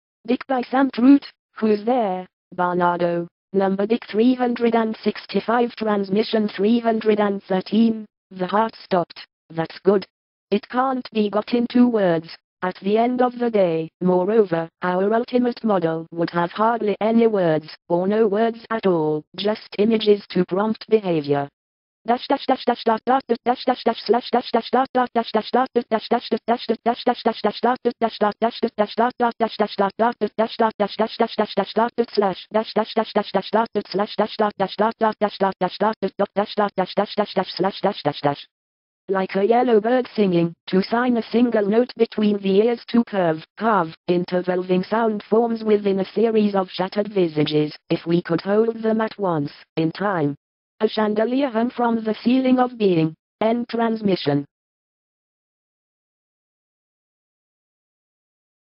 DICK 313
This is one of 365 transmissions (22.Nov.12 - 22.Nov.13) of DICK, a book by Sam Truitt. For more see DICK: An Oblique Kennedy Conspiracy Countdown.